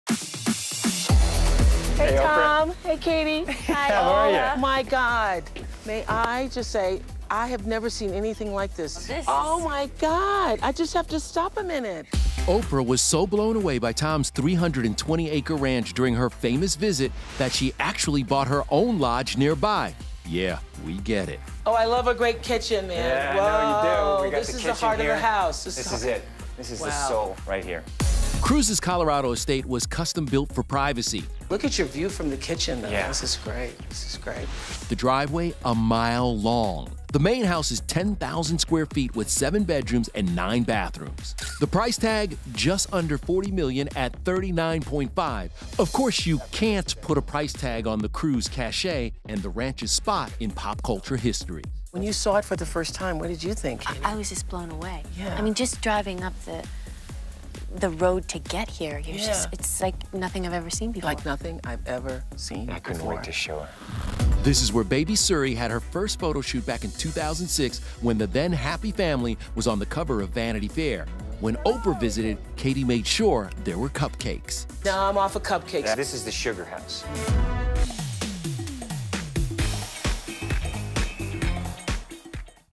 Tom Cruise Selling His $39.5 Million Colorado Ranch: See Inside the Massive Home
ET is taking a look inside the 320-acre ranch in Telluride, Colorado, that Cruise initially purchased in 2014.